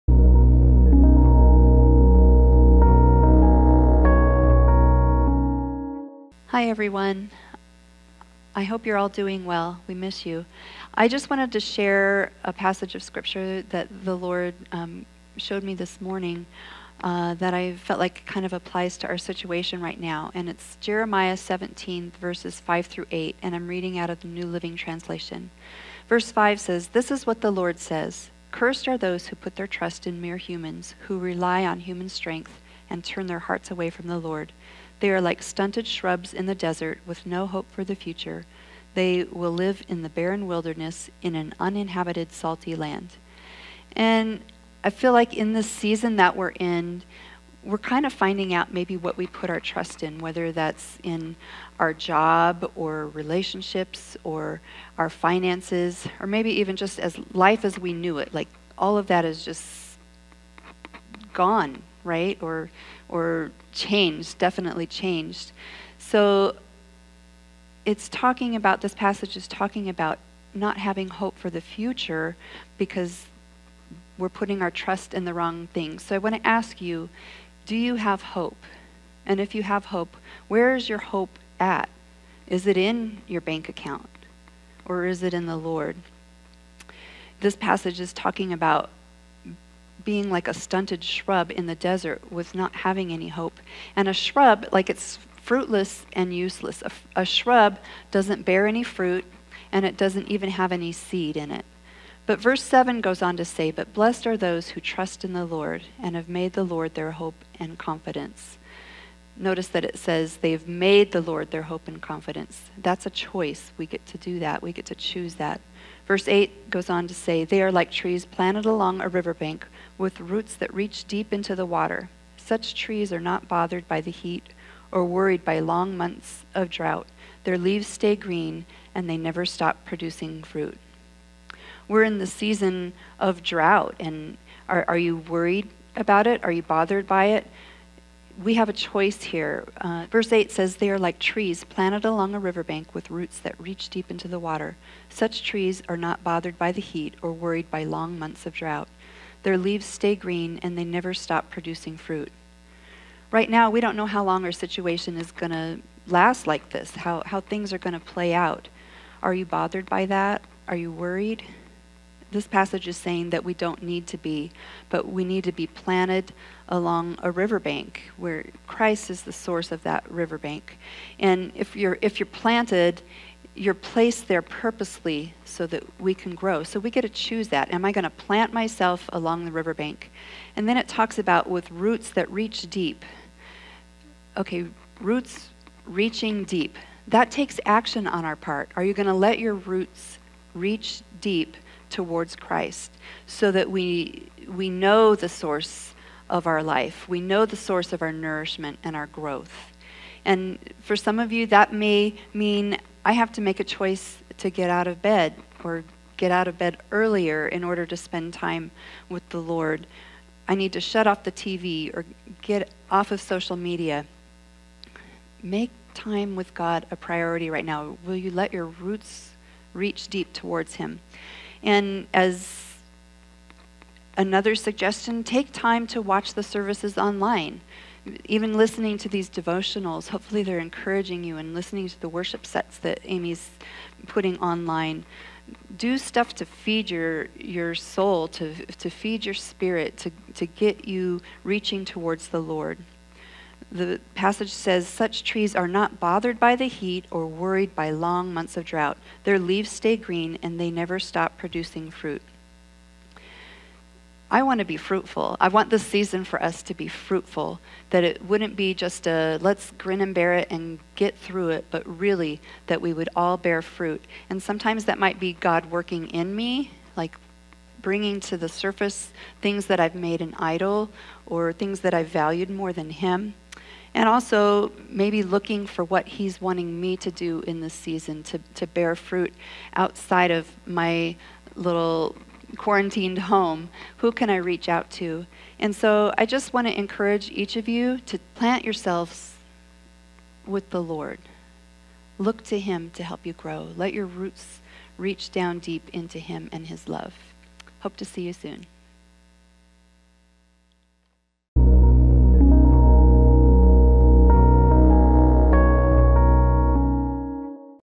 Planted in the Lord, A Short Devotional by Pastor Lisa Wright
Do you have hope? What is your hope?